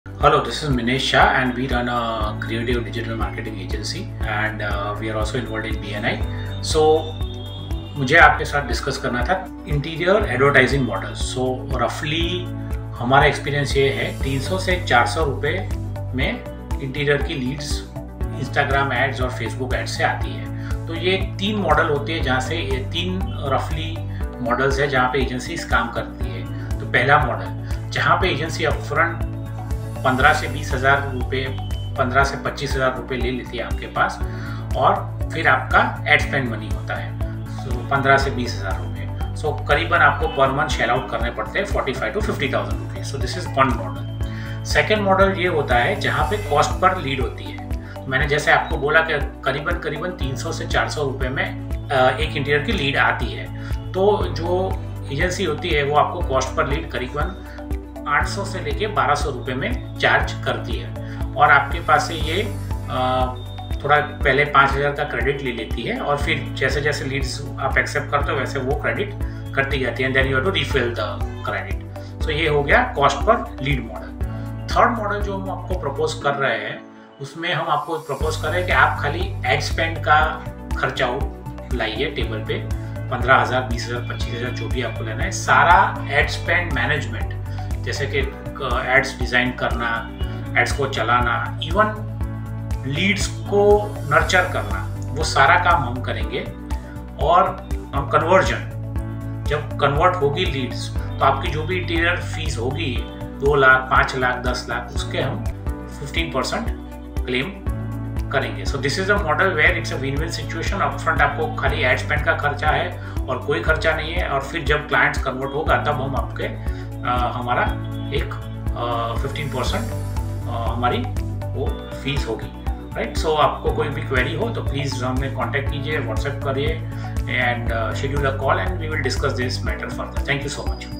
Transform Your Interior Marketing: Pay for Results Only | Instagram & Google Ads | BNI Members
In this video, we unveil a revolutionary approach to interior marketing that allows you to pay for results only—no upfront costs involved!

Model:

Interior Lead Generation & Marketing by Instagram/Google Ads :

Service scope: Lead generation + Lead nurturing, calling, and qualifying leads.

Usual charges in ₹

Existing Model in the Market

Agency charges: - ₹ 10k-25k
Ad spend:          - ₹ 5k-20k
Total spend :      - ₹ 20k-50k (Approx)

New Model  : *Our Proposed Model*

Less or No upfront agency cost & more stake in each other
Ad spend: 5k-25k in your account depending on your budget
NO OTHER CHARGES

*15 % on client conversion.

Benefits of Our Model :
— No charges upfront
— Less investment
— We will be involved in nurturing and even help you close the client
— We manage your ad and Instagram account.